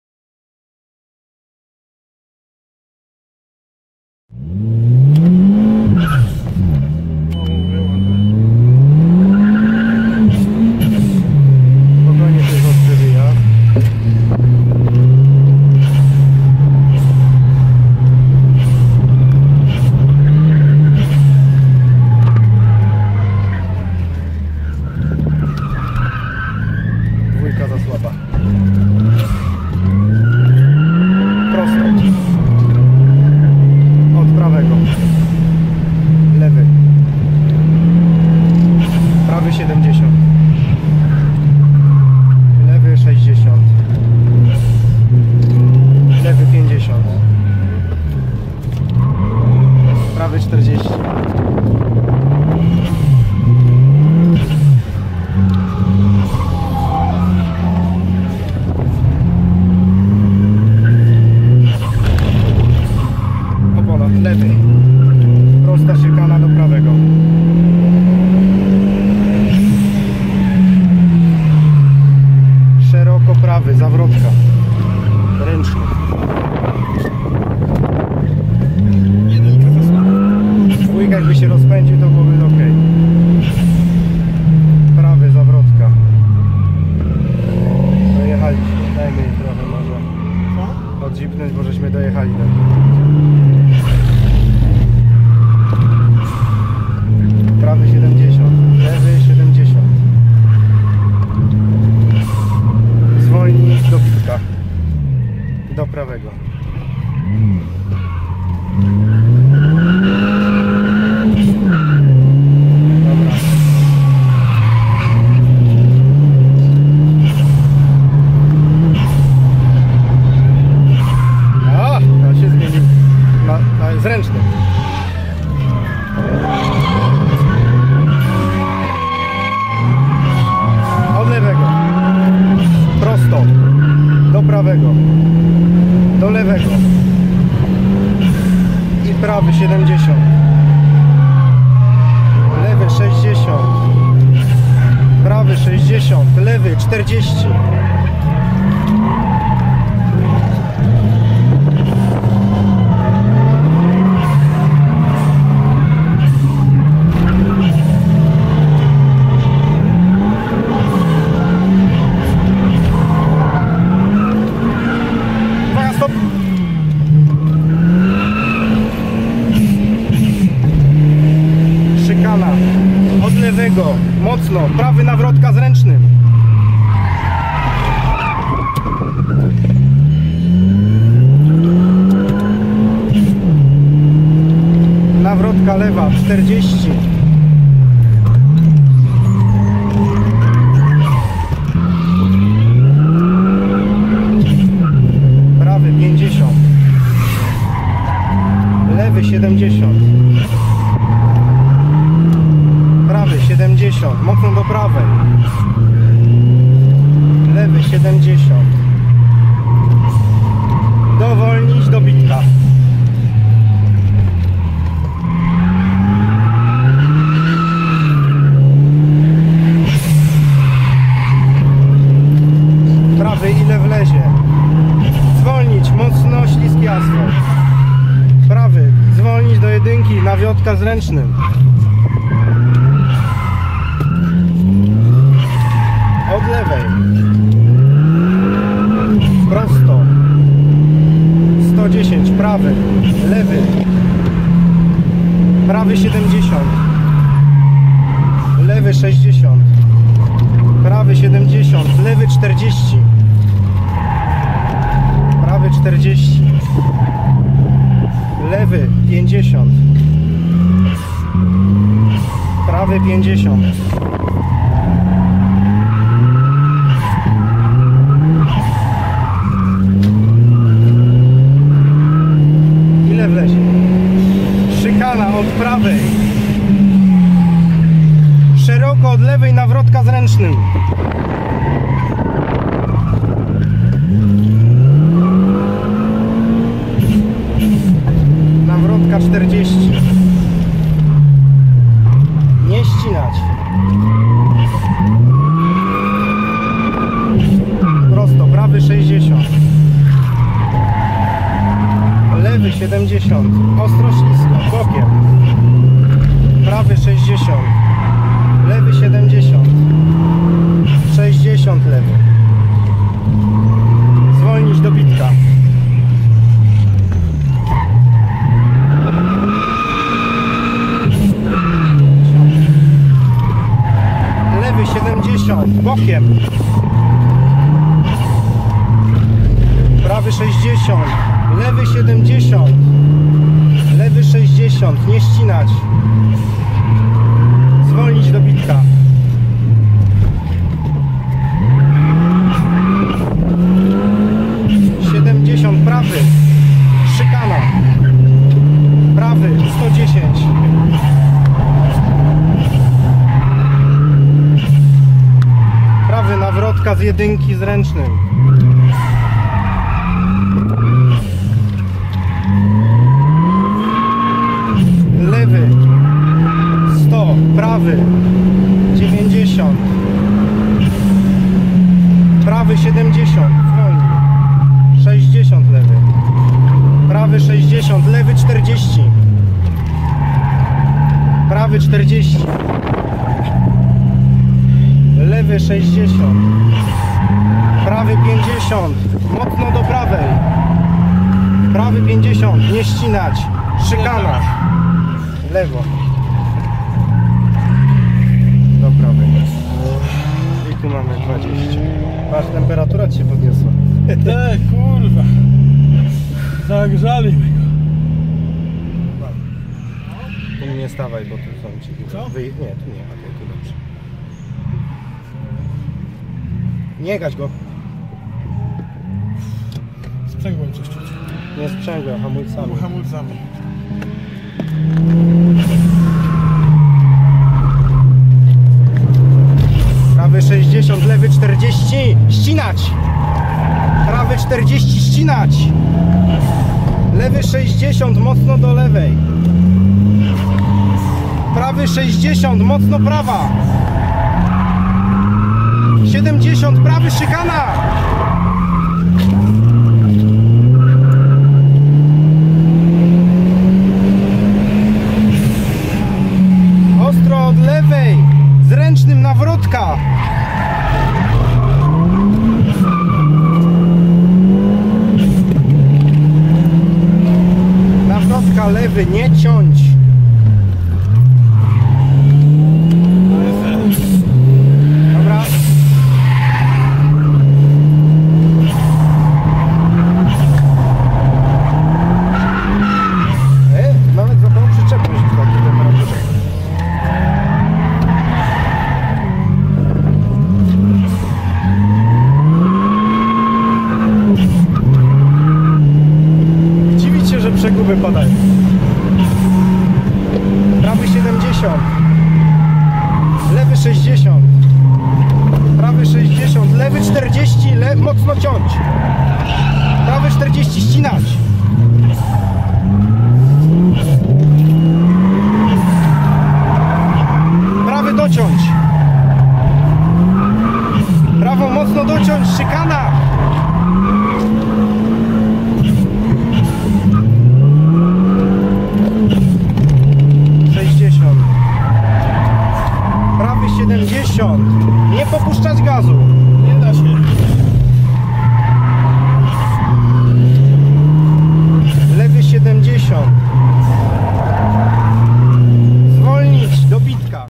Tor Kielce Skoda Octavia 1.8t Tomasz i Marcin Szatan
Traning przed Kielecki SuperOES na torze w Kielcach.
Załoga Tomasz i Marcin Szatan
Skoda Octavia 1.8T